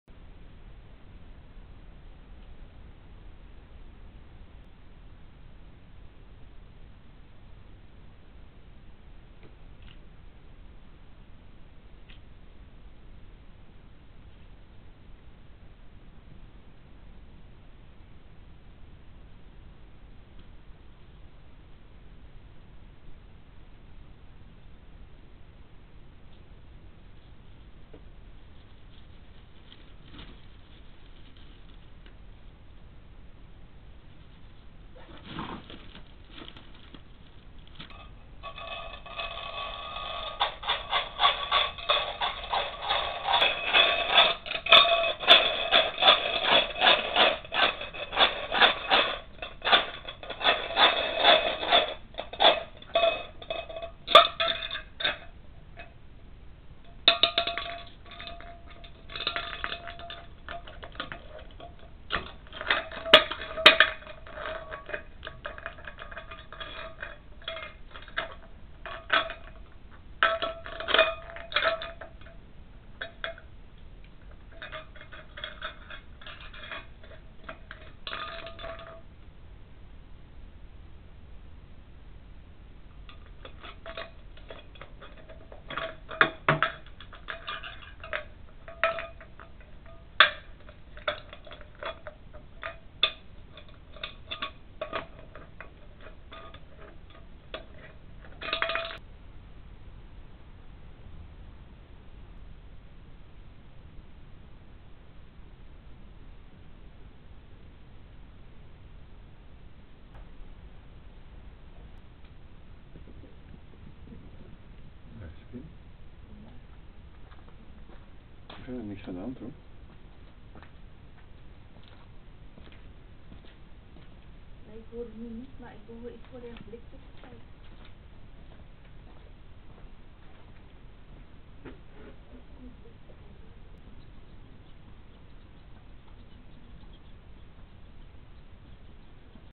Egel maakt herrie met leeg blikje, weet ongezien te blijven
's Nachts rond een uur of vier kregen we een egel op bezoek die het hele gezin wist te wekken met zijn activiteiten. Pim de kat had hem in de gaten maar de heer des huizes had helaas geen infraroodogen.